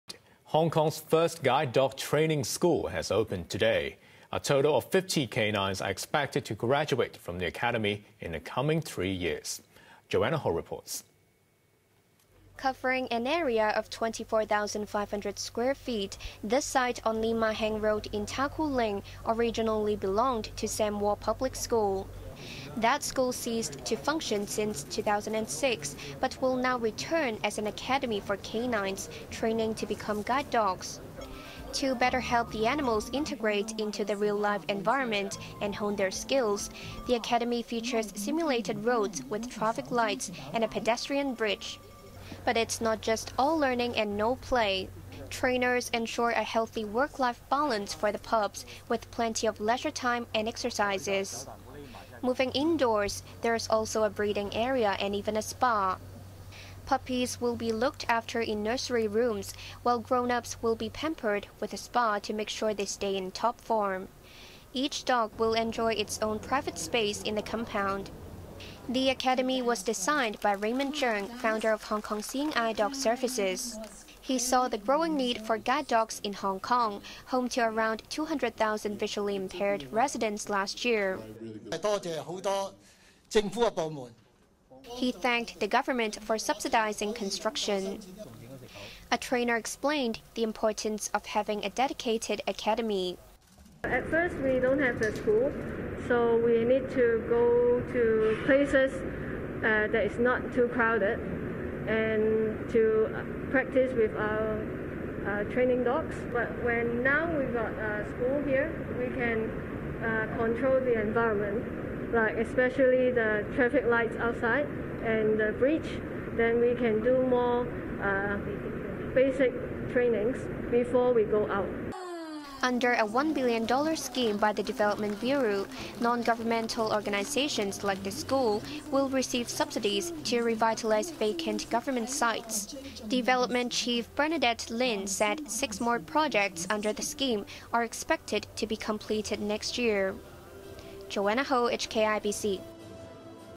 HK Welcomes First Guide Dog Training School | HKIBC News
Hong Kong's first guide dog training school has opened today.

A total of 50 canines are expected to graduate from the academy in the coming three years.